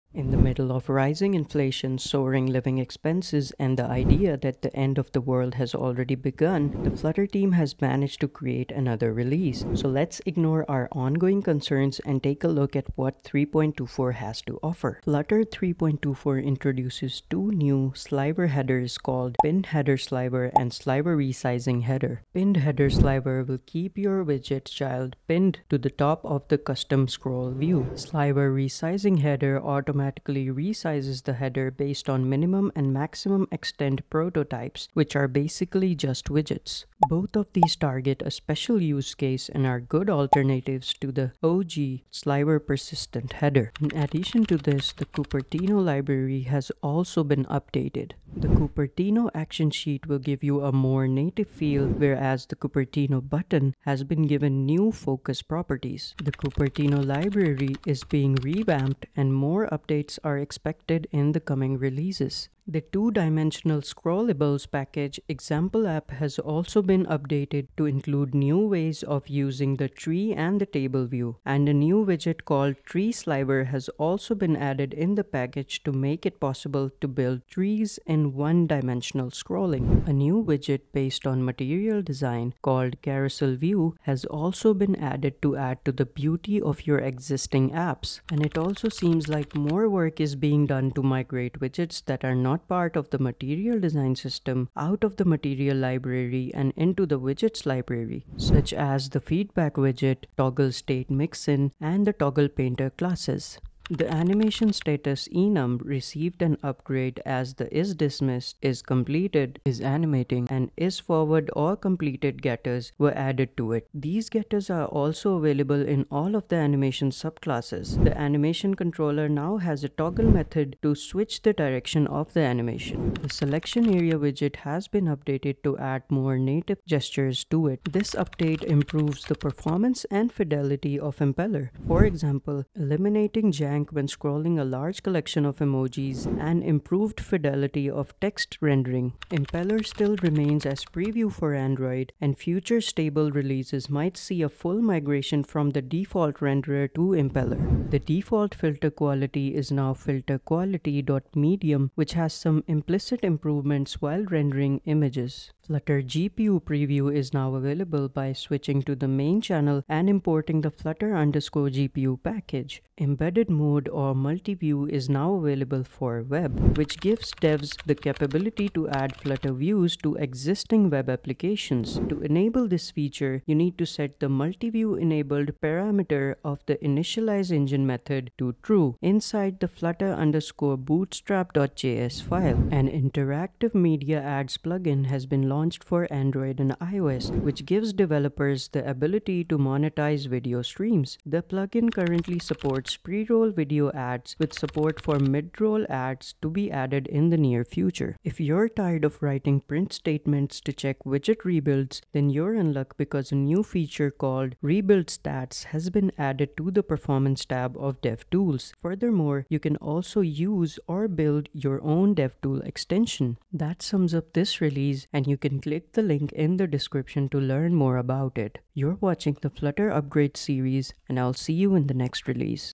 What's new in Flutter 3.24 | Flutter 2024 Updates
In this video, we're going to quickly go over what's new in Flutter 3.24 and some of its key updates.
Future prospects of Flutter continue to look even promising with performance upgrades to Flutter_GPU and Impeller. Also, a lot of new UI stuff has been added / tweaked in this release!

Timestamps:
Intro 0:00 - 0:15
SliverHeaders 0:15 - 0:42
Cupertino updates 0:42 - 1:01
Tree/TableView 1:01 - 1:19
CarouselView 1:19 - 1:27
Widget Migrations 1:27 - 1:43
Animation updates 1:43 - 2:02
SelectionArea 2:02 - 2:07
Impeller 2:07 - 2:27
Image Filter 2:27 - 2:35
Flutter GPU 2:35 - 2:42
Web 2:42 - 3:02
Media Ads 3:02 - 3:18
DevTools 3:18 - 3:33
Subscribe please 3:33 - 3:42